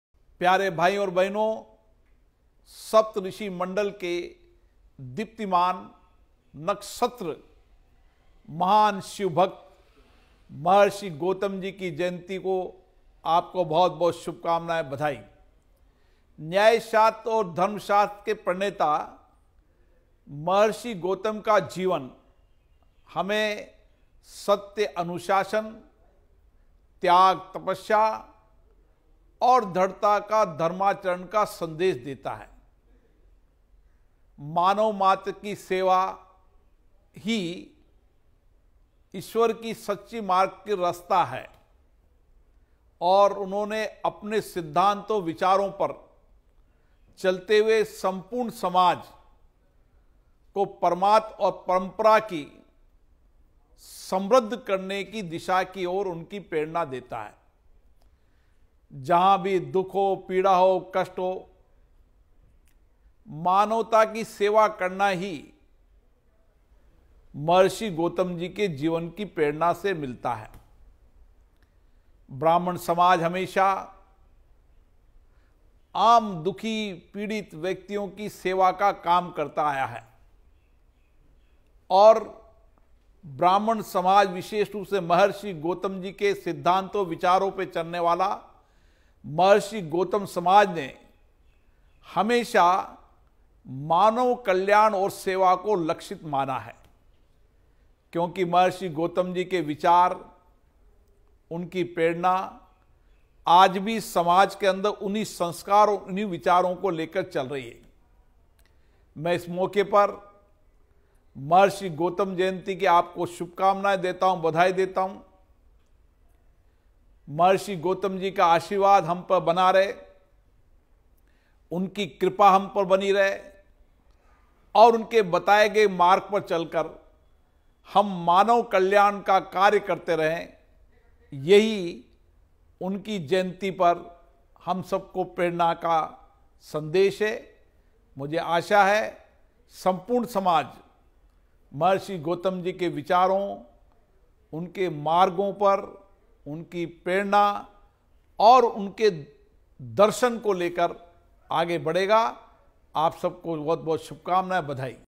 Maharishi Gautam's birth anniversary - A message by speaker
Mar.22.2023

सप्तर्षि मंडल के दीप्तिमान नक्षत्र, महान शिवभक्त अक्षपाद महर्षि गौतम जी की जयंती की आप सभी को हार्दिक शुभकामनाएं। न्याय शास्त्र और धर्म शास्त्र के प्रणेता महर्षि गौतम की कृपा हम सभी पर बनी रहे। उनके पुण्य आशीर्वाद से विश्व कल्याण हो, यही प्रार्थना है।